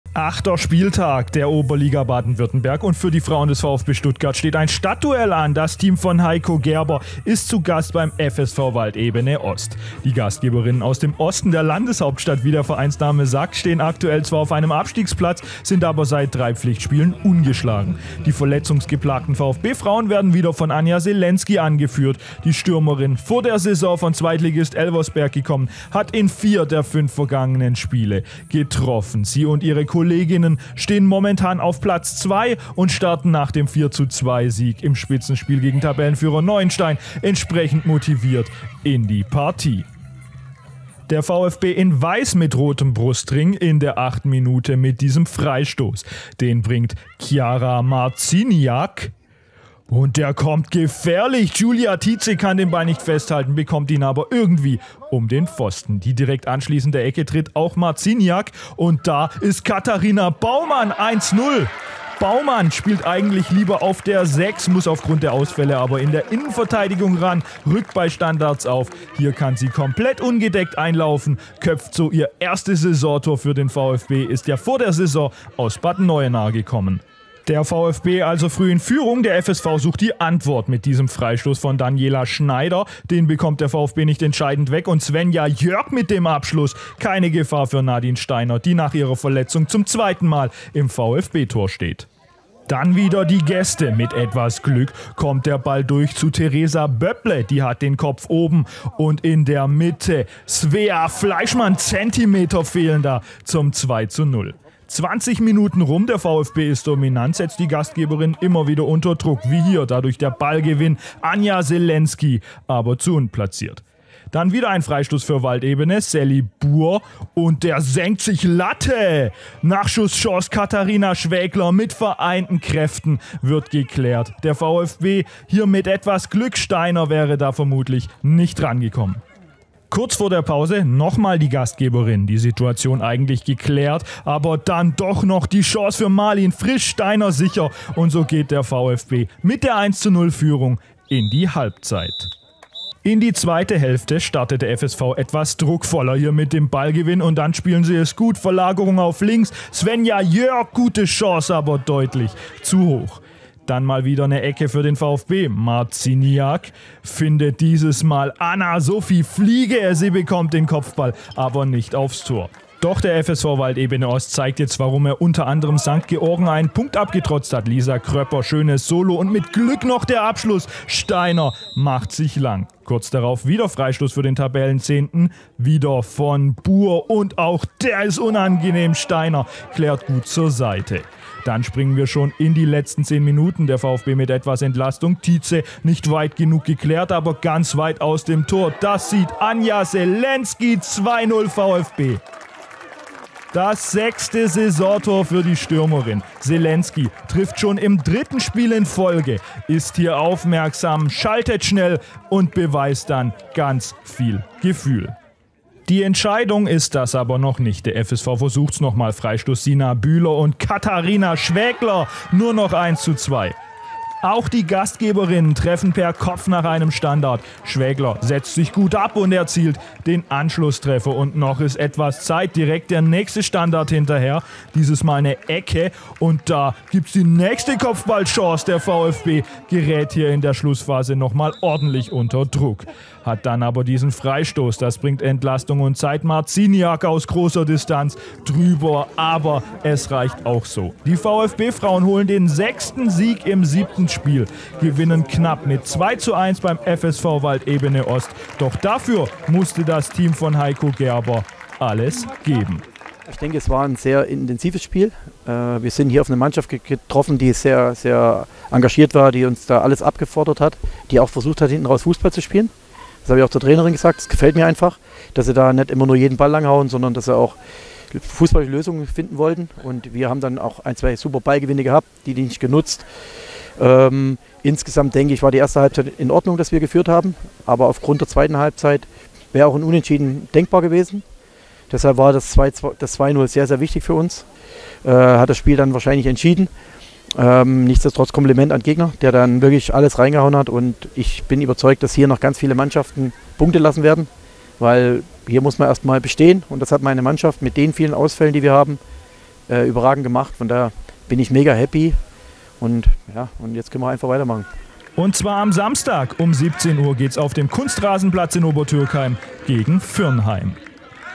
Highlights: FSV Waldebene Stuttgart Ost - VfB-Frauen
Am Wochenende haben die VfB-Frauen das Stadt-Derby in der Oberliga gegen FSV Waldebene Stuttgart Ost erfolgreich für sich entschieden. Katharina Baumann und Anja Selensky erzielten die Treffer zum 2:1-Auswärtssieg.

Die Tore:
00:00 Start
00:54 1:0 Katharina Baumann
03:01 2:0 Anja Selensky

VfBeSports:  @VfB eSports